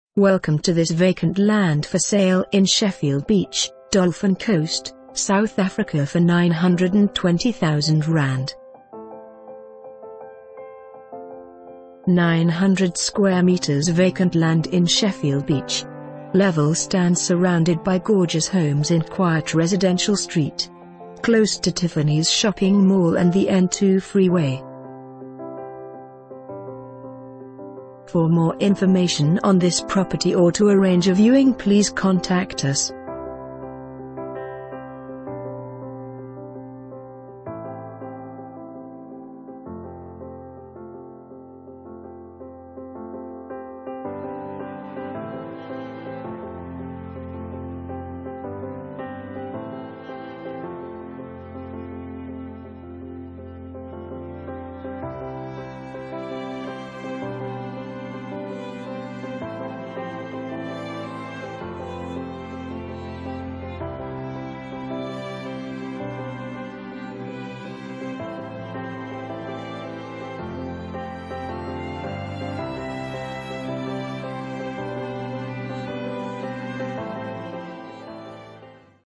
Vacant Land For Sale in Sheffield Beach, Dolphin Coast, South Africa for ZAR 920,000...
900m2 vacant land in Sheffield Beach.

Level stand surrounded by gorgeous homes in quiet residential street.  Close to Tiffany's shopping mall and the N2 freeway.